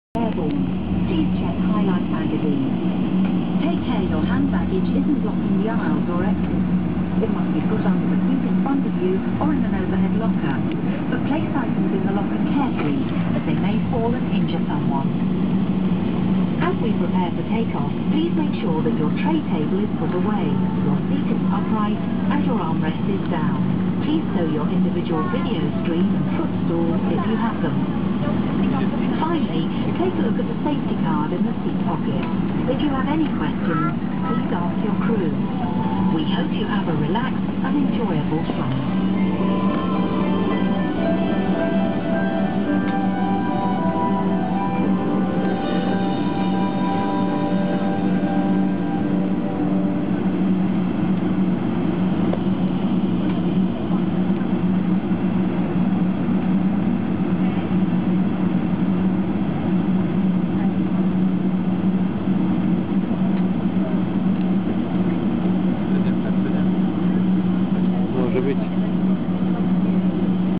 Cruising on the Airport Gatwick Boeing 777-200 BA
Moving to the runway with British Airways Boeing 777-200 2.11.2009 afternoon.